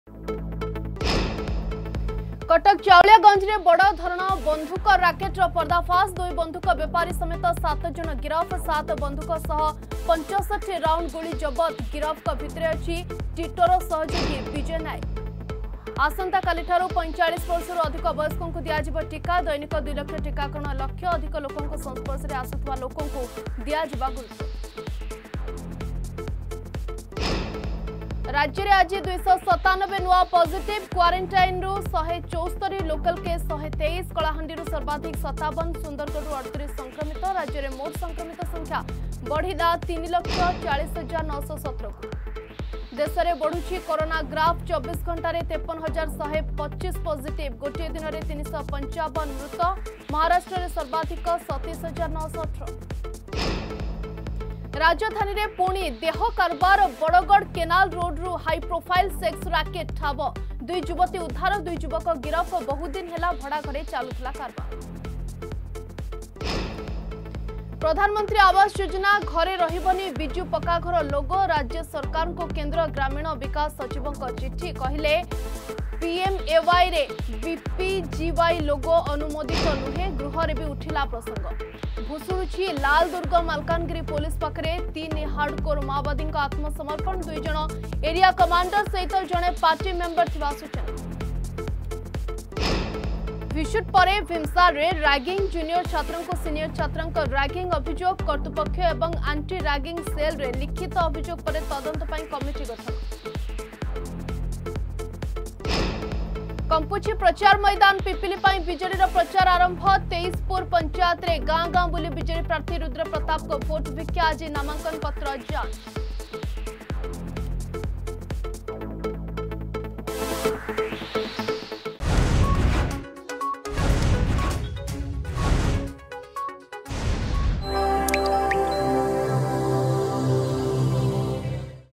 Headlines 4pm
କଟକ ଚାଉଳିଆଗଞ୍ଜରେ ବଡଧରଣ ବନ୍ଧୁକ ରାକେଟର ପର୍ଦ୍ଦାଫାଶ.
ଆସନ୍ତାକାଲି ଠାରୁ 45 ବର୍ଷରୁ ଅଧିକ ବୟସ୍କଙ୍କୁ ଦିଆଯିବ ଟୀକା.
ରାଜ୍ୟରେ ଆଜି 297 ନୂଆ ପଜିଟିଭ...
ଦେଶରେ ବଢ଼ୁଛି କୋରୋନା ଗ୍ରାଫ...
ରାଜଧାନୀରେ ପୁଣି ଦେହ କାରବାର...
ପ୍ରଧାନମନ୍ତ୍ରୀ ଆବାସ ଯୋଜନା ଘରେ ରହିବନି ବିଜୁ ପକ୍କାଘର ଲୋଗୋ...
 ଭୁଶୁଡୁଛି ଲାଲ ଦୁର୍ଗ...
ଭିସୁଟ ପରେ ଭୀମସାରରେ ରାଗିଂ...
କମ୍ପୁଛି ପ୍ରଚାର ମୈଦାନ...